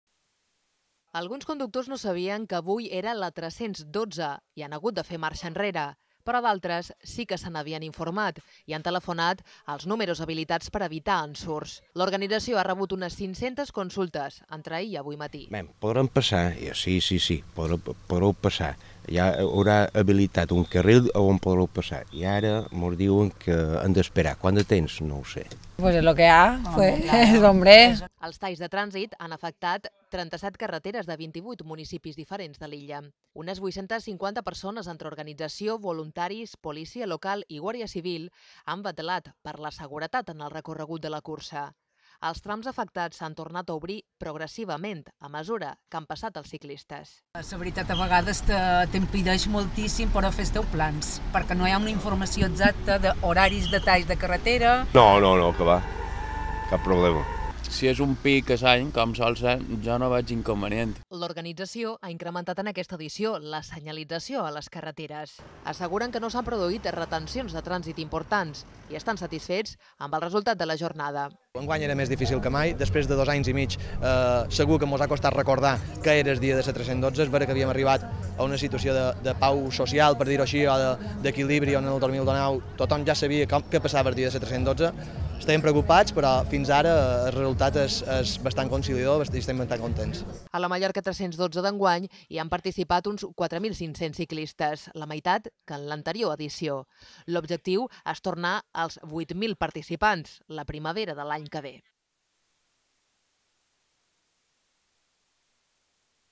37 carreteres tallades per mor de la curs ciclista Mallorca 312
A la Mallorca 312 d'enguany hi han participat uns 4.500 ciclistes, la meitat que en l'anterior edició